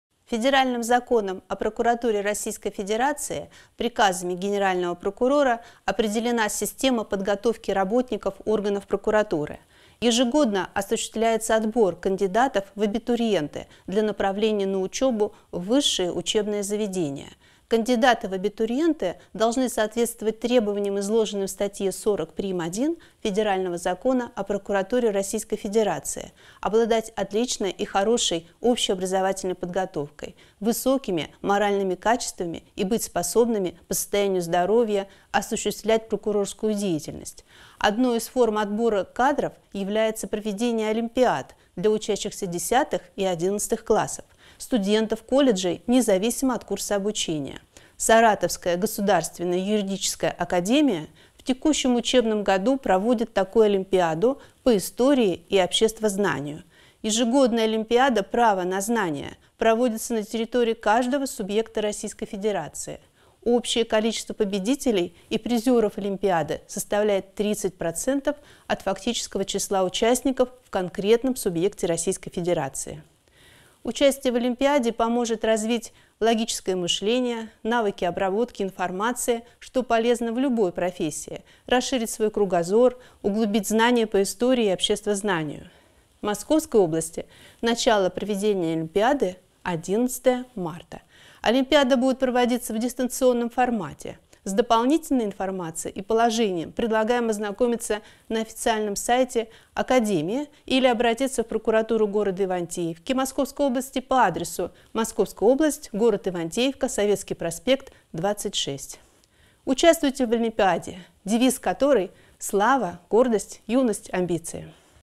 Право на знание. Ивантеевским школьникам предложили принять участие в олимпиаде
Информация для учеников 10-х, 11-х классов и студентов колледжей. Ивантеевская прокуратура предлагает ребятам поучаствовать в Олимпиаде «Право на знание» и получить возможность стать абитуриентом одного из высших учебных заведений.